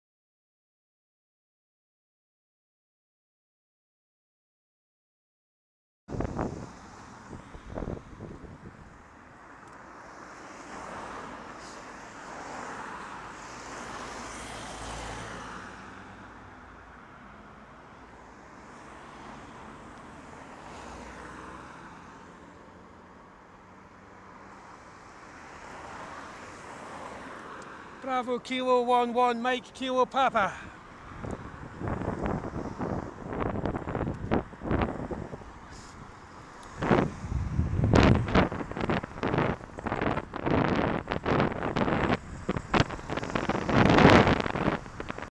BK11MKP runs red light
Never attempted to stop, never braked at all.